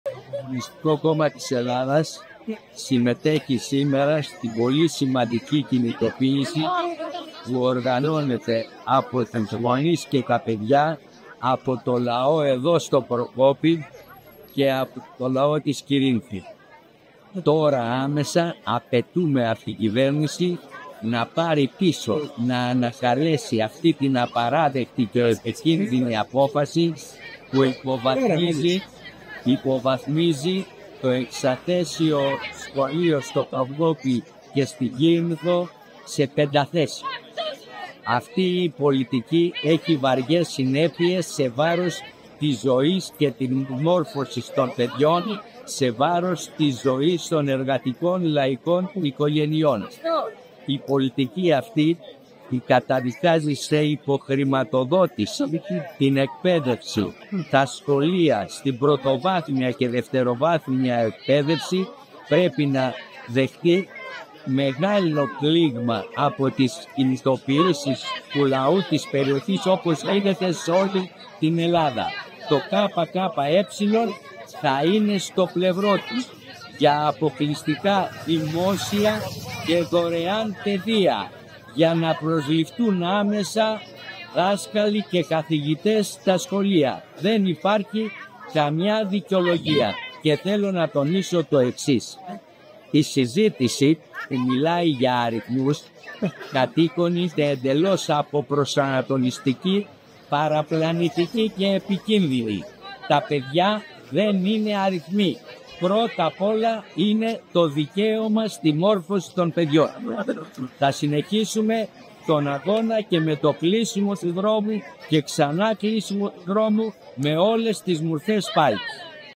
Ο Γ. Μαρίνος σε κινητοποίηση για τα σχολεία στην Εύβοια
Στην κινητοποίηση των γονέων, των μαθητών και του λαού ενάντια στην υποβάθμιση των Σημοτικών Σχολείων Προκοπίου και Κηρίνθου στη Βόρεια Εύβοια παραβρέθηκε ο Γιώργος Μαρίνος, μέλος του ΠΓ της ΚΕ του ΚΚΕ και βουλευτής Εύβοιας, εκφράζοντας τη στήριξη του ΚΚΕ στον αγώνα που δίνουν.

Χαρακτηρίζοντας ως ιδιαίτερα σημαντική κινητοποίηση και το κλείσιμο του δρόμου, ο Γιώργος Μαρίνος, σε δήλωσή του, αναφέρει:

«Τώρα, άμεσα, απαιτούμε από την κυβέρνηση να ανακαλέσει αυτή την απαράδεκτη και επικίνδυνη απόφαση που υποβαθμίζει τα Δημοτικά Σχολεία Προκοπίου και Κηρίνθου. Αυτή η πολιτική έχει σοβαρές συνέπειες σε βάρος της ζωής και της μόρφωσης των παιδιών, σε βάρος της ζωής των εργατικών - λαϊκών οικογενειών. Η πολιτική αυτή που καταδικάζει σε υποχρηματοδότηση την Εκπαίδευση, τα σχολεία σε Πρωτοβάθμια και Δευτεροβάθμια Εκπαίδευση, πρέπει να δεχθεί μεγάλο πλήγμα από τις κινητοποιήσεις του λαού της περιοχής, όπως γίνεται σε όλη την Ελλάδα».

Τονίζοντας ότι το ΚΚΕ θα είναι στο πλευρό τους παλεύοντας για «αποκλειστικά δημόσια και δωρεάν Παιδεία, για να προσληφθούν άμεσα δάσκαλοι και καθηγητές στα σχολεία», ο Γιώργος  Μαρίνος υπογράμμισε ότι «δεν υπάρχει καμία δικαιολογία» για την κατάσταση αυτή και καταλήγοντας επισήμανε:

«Θέλω να τονίσω το εξής: Η συζήτηση που μιλάει για αριθμούς κατοίκων και μαθητών είναι αποπροσανατολιστική, παραπλανητική και επικίνδυνη. Τα παιδιά δεν είναι αριθμοί. Πρώτο από όλα είναι το δικαίωμα των παιδιών στη μόρφωση. Θα συνεχίσουμε τον αγώνα και με κλείσιμο του δρόμου και με όλες τις μορφές πάλης».